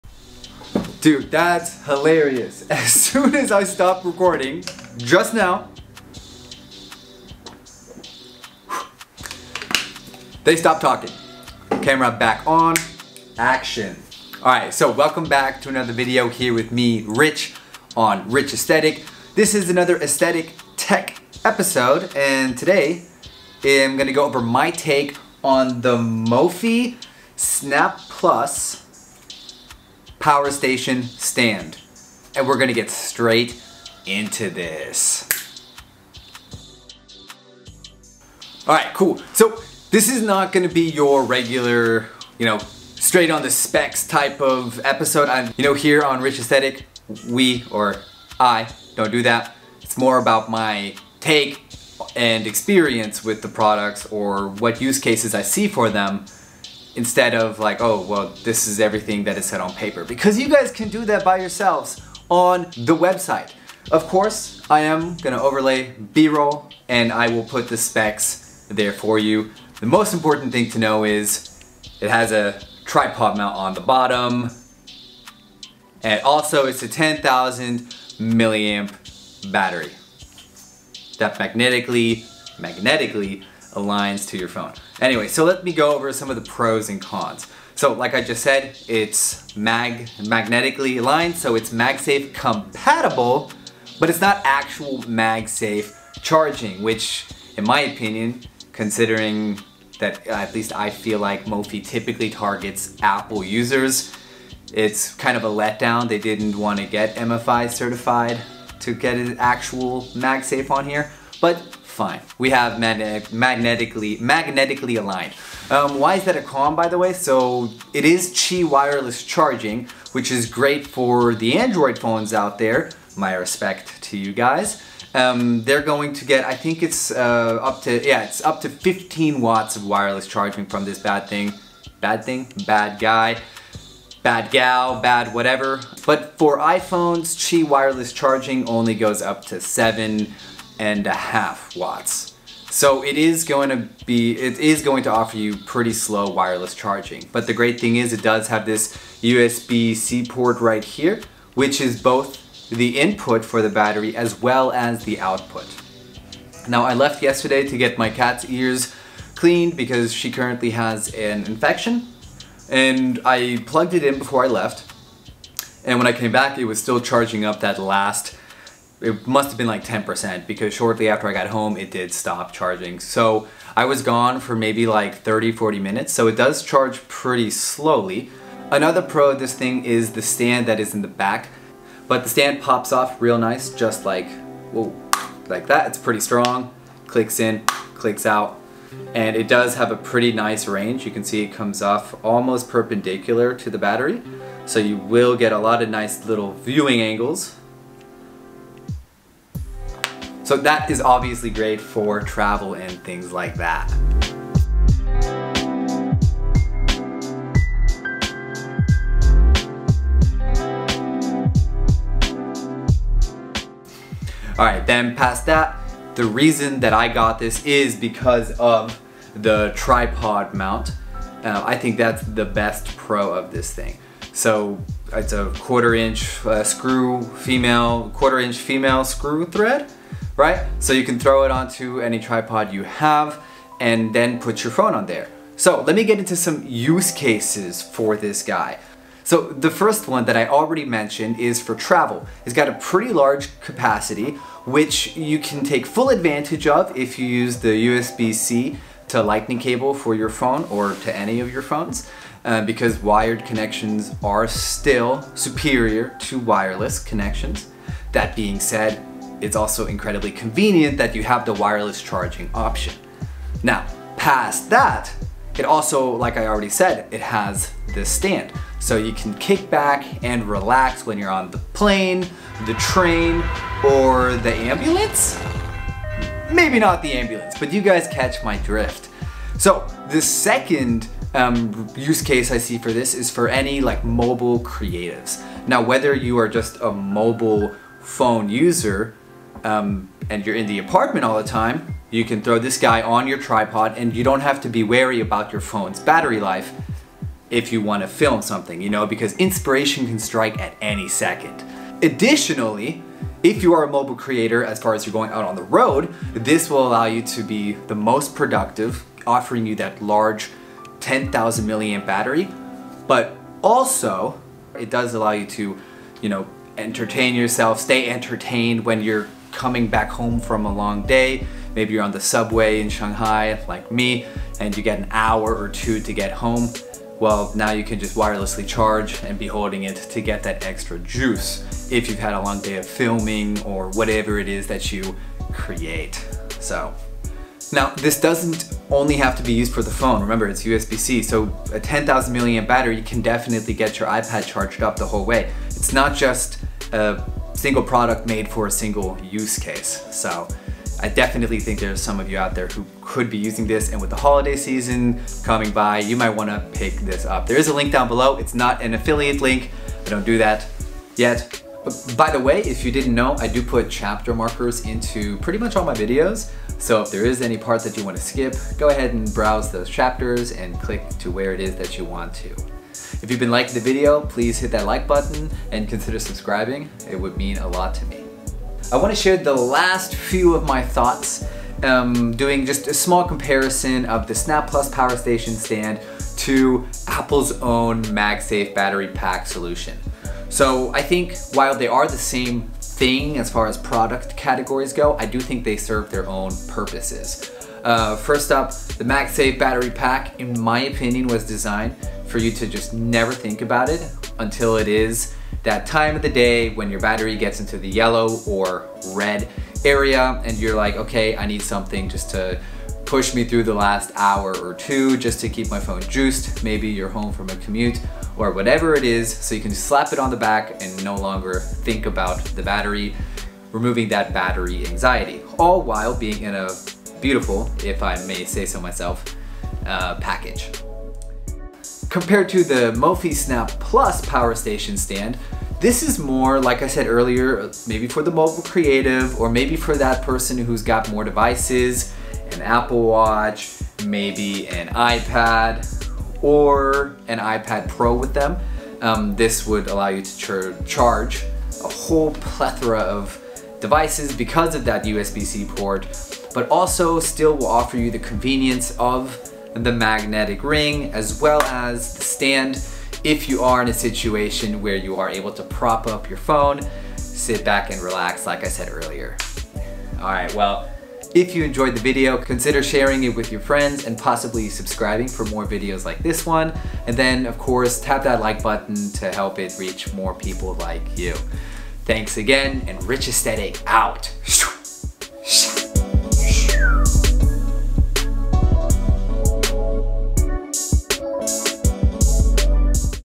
Aesthetic Tech 12 | Mophie snap+ powerstation stand
✌️ Really like this battery pack and hope y'all do to! comment what you'll be using it for
🤜 This is my review of the Mophie snap+ powerstation stand

chapters:
00:00 - intro
00:49 - snap+ box contents
01:28 - Pros and Cons - MagSafe *Compatible*
02:27 - Pros and Cons - In/Out USB C Charging Port
03:37 - Pros and Cons - Tripod Mount
03:55 - Use Cases
06:18 - Comparing snap+ powerstation stand to MagSafe Battery Pack
06:40 - MagSafe Battery Pack is for …
07:11 - snap + powerstation stand is for…